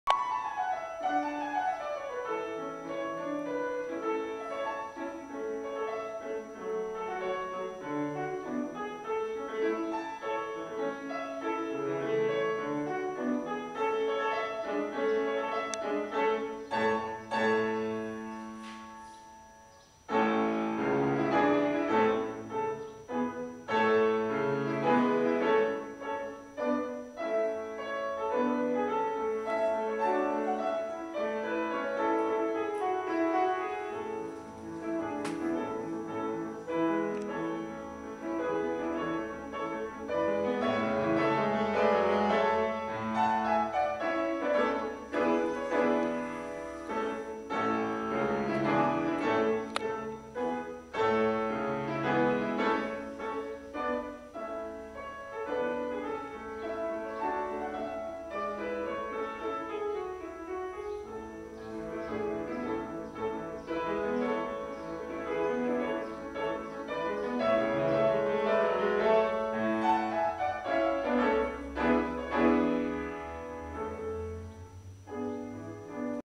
Unity Temple
a building that feels like a music or jewerly box, listening to  a piano practice by chance.